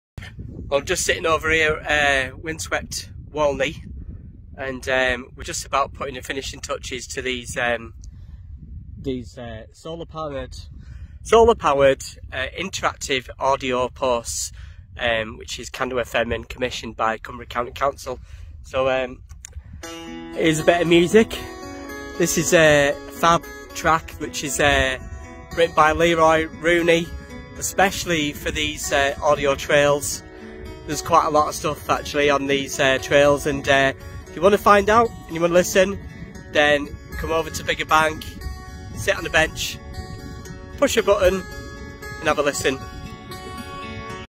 Interactive Audio Posts
Interactive Audio trails on Bigger Bank by CandoFM commissioned by Cumbria County Council